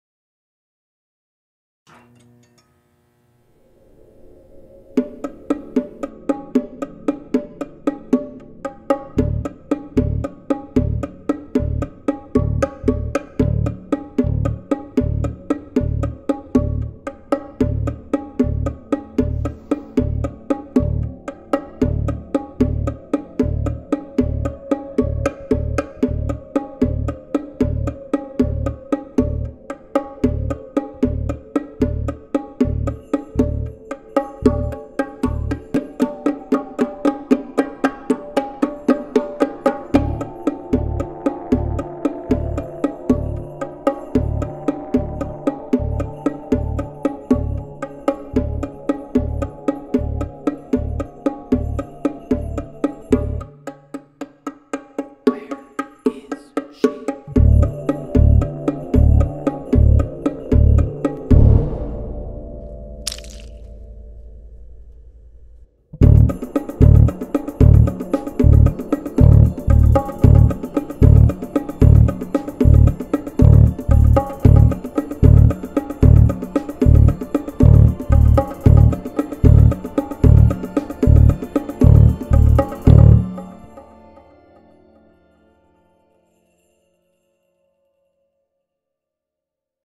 why are you just watching
a short experimental film made for my high school cinema studies class